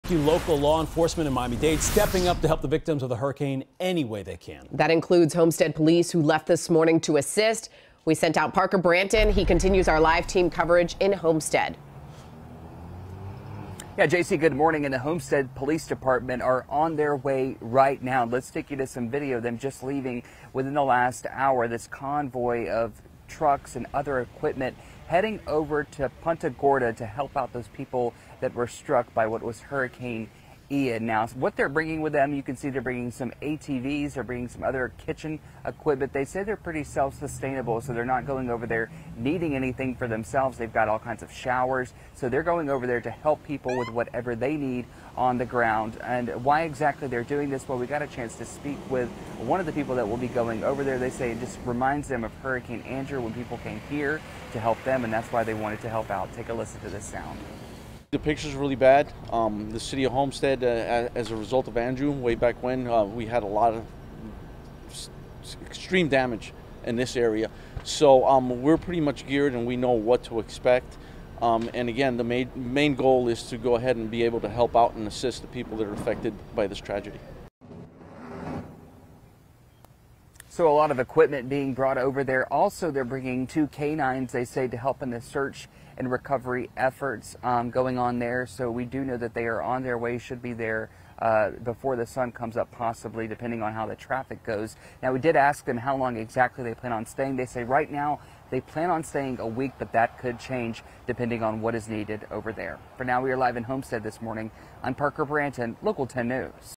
Homestead police sends team to Punta Gorda after Hurricane Ian
A team from the Homestead Police Department is on their way to Punta Gorda after Hurricane Ian devastated the area.